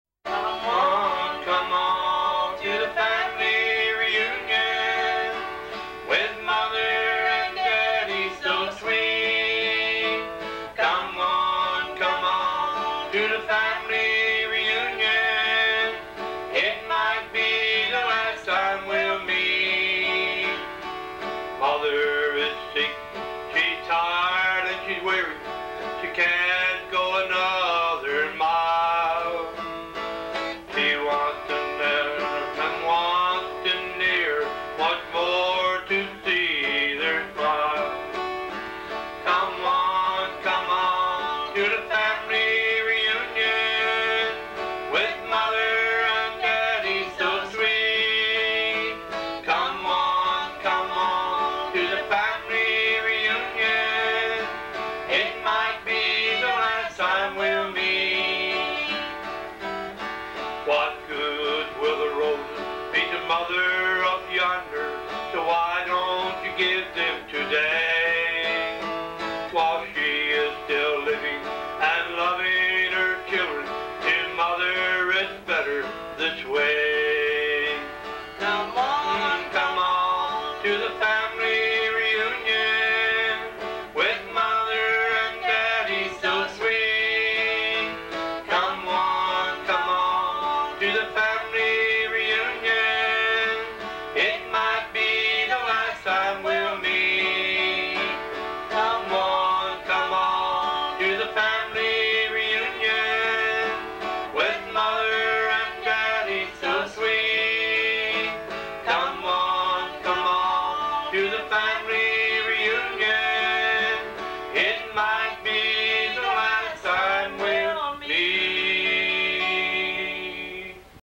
Family Reunion
Hymn sung by Priscilla and Roland Cooper, Twillingate, Newfoundland/Labrador.